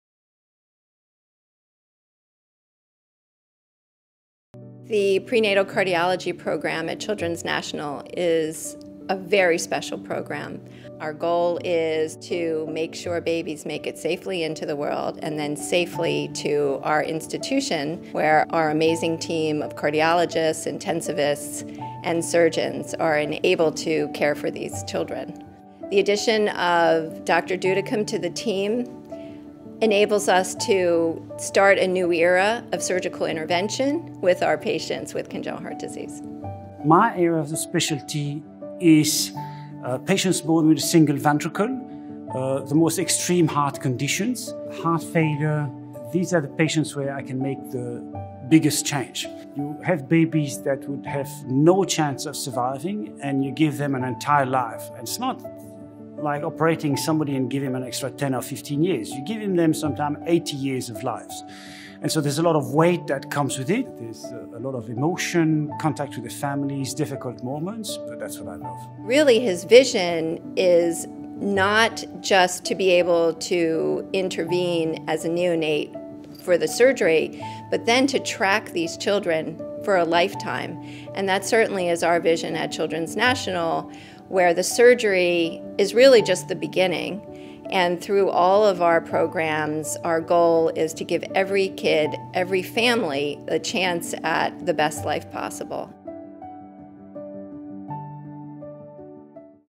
The Prenatal Cardiology Program Welcomes Cardiac Surgeon Yves d'Udekem, M.D., Ph.D.
The Children's National Prenatal Cardiology Program welcomes expert pediatric cardiac surgeon Yves d'Udekem, M.D., Ph.D. The Prenatal Cardiology Program takes care of babies before they're born and helps transition them into childhood. With expert cardiologists, intensivists and cardiac surgeons, we work to give children a chance at the best life possible.